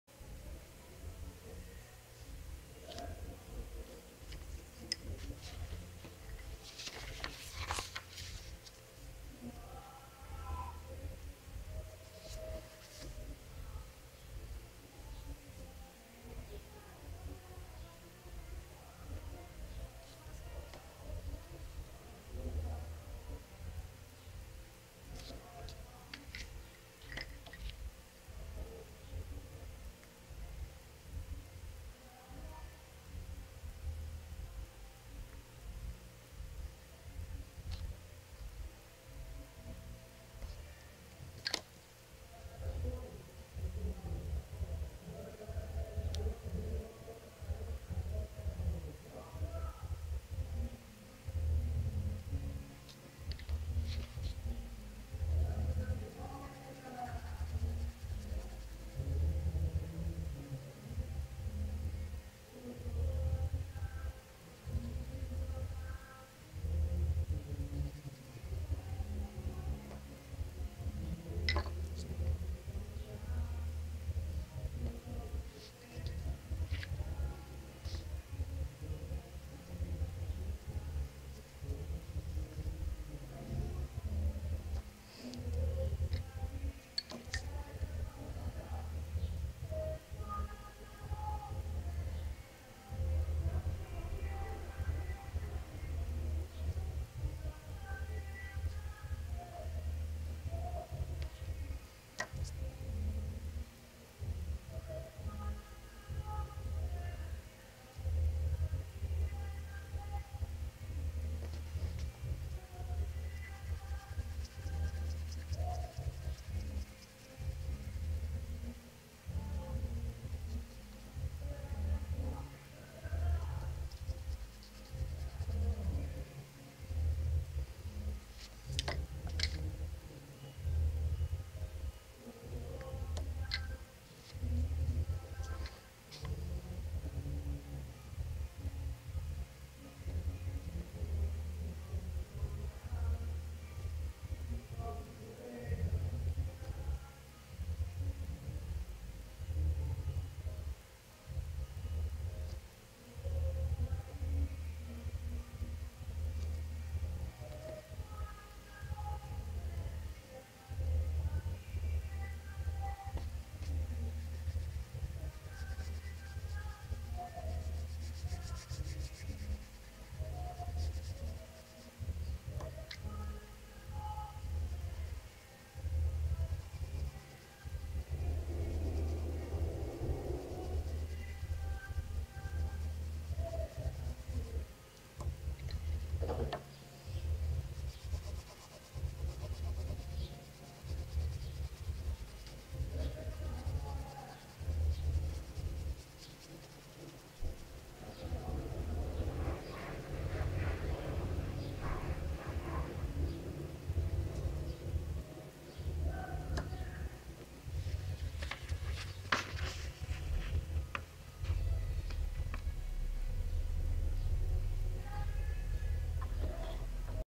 Moda! Baby looks gatinhos!!
Baby looks pintadinha com cara de gatinhos, usando EcoLápis de Cor Triangular da Faber-Castell um arraso, e sempre na moda neste verão, espero que tenha gostado se inscreva e compartilhe para me ajudar, que Deus abençõe a todos.